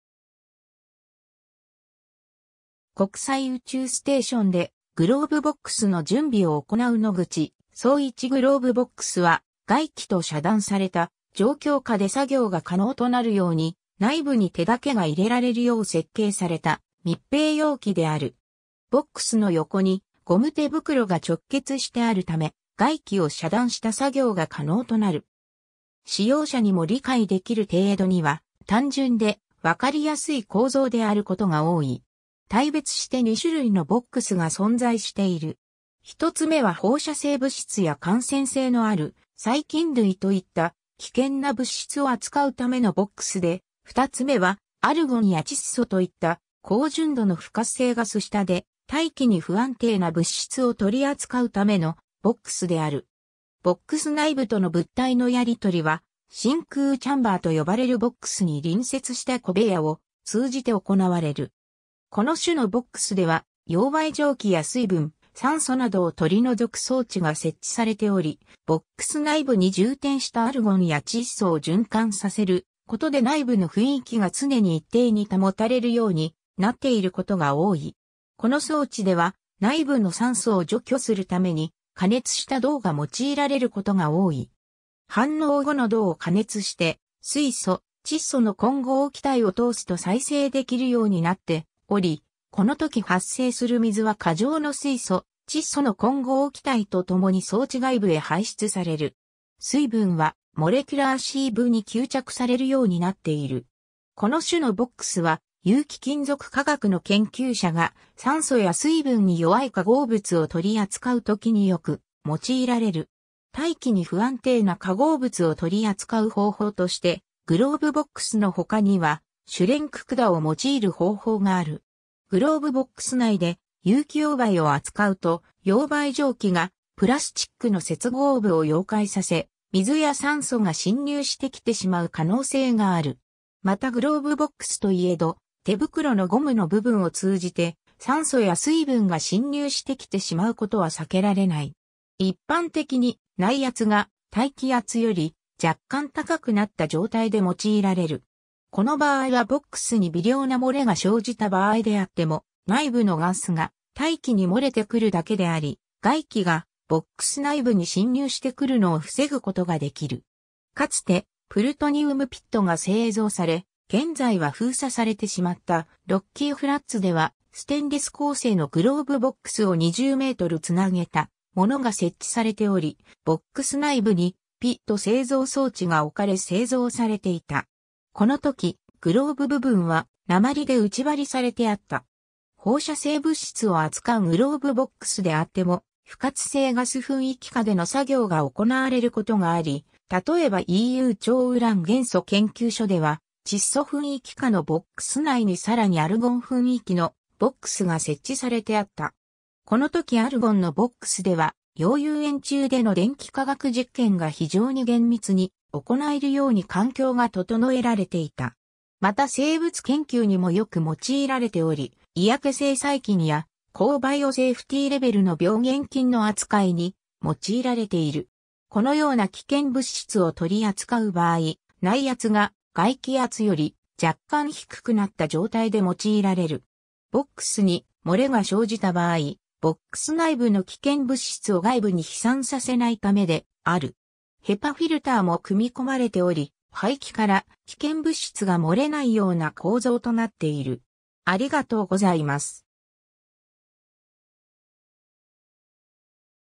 グローブボックス
国際宇宙ステーションでグローブボックスの準備を行う野口聡一 グローブボックス（英: glovebox）は、外気と遮断された状況下で作業が可能となるように、内部に手だけが入れられるよう設計された密閉容器である。
ボックスの横にゴム手袋（グローブ）が直結してあるため、外気を遮断した作業が可能となる。
使用者にも理解できる程度には、単純で分かりやすい構造であることが多い。
 大別して2種類のボックスが存在している。
1つめは放射性物質や感染性のある細菌類といった危険な物質を扱うためのボックスで、2つめはアルゴンや窒素といった高純度の不活性ガス下で、大気に不安定な物質を取り扱うためのボックスである。
ボックス内部との物体のやり取りは、真空チャンバーと呼ばれるボックスに隣接した小部屋を通じて行われる。
 この種のボックスでは溶媒蒸気や水分、酸素などを取り除く装置が設置されており、ボックス内部に充填したアルゴンや窒素を循環させることで内部の雰囲気が常に一定に保たれるようになっていることが多い。
この装置では内部の酸素を除去するために、加熱した銅が用いられることが多い。
反応後の銅を加熱して水素/窒素の混合気体を通すと再生できるようになっており、このとき発生する水は過剰の水素/窒素の混合気体と共に装置外部へ排出される。
水分はモレキュラーシーブに吸着されるようになっている。
この種のボックスは、有機金属化学の研究者が酸素や水分に弱い化合物を取り扱うときによく用いられる。
 大気に不安定な化合物を取り扱う方法として、グローブボックスの他にはシュレンク管を用いる方法がある。
グローブボックス内で有機溶媒を扱うと、溶媒蒸気がプラスチックの接合部を溶解させ、水や酸素が浸入してきてしまう可能性がある。
またグローブボックスといえど、手袋のゴムの部分を通じて酸素や水分が浸入してきてしまうことは避けられない。
 一般的に、内圧が大気圧より若干高くなった状態で用いられる。
この場合はボックスに微量な漏れが生じた場合であっても内部のガスが大気に漏れてくるだけであり、外気がボックス内部に浸入してくるのを防ぐことができる。
 かつてプルトニウムピットが製造され、現在は封鎖されてしまったロッキーフラッツでは、ステンレス鋼製のグローブボックスを20メートル繋げたものが設置されており、ボックス内部にピット製造装置が置かれ製造されていた。
このときグローブ部分は鉛で内張されてあった。
放射性物質を扱うグローブボックスであっても不活性ガス雰囲気下での作業が行われることがあり、例えばEU超ウラン元素研究所 (ITU) では窒素雰囲気下のボックス内に更にアルゴン雰囲気のボックスが設置されてあった。
このときアルゴンのボックスでは、溶融塩中での電気化学実験が非常に厳密に行えるように環境が整えられていた。
 また生物研究にもよく用いられており、嫌気性細菌や高バイオセーフティーレベルの病原菌の扱いに用いられている。
 このような危険物質を取り扱う場合、内圧が外気圧より若干低くなった状態で用いられる。
ボックスに漏れが生じた場合、ボックス内部の危険物質を外部に飛散させないためである。
HEPAフィルターも組み込まれており、排気から危険物質が漏れないような構造となっている。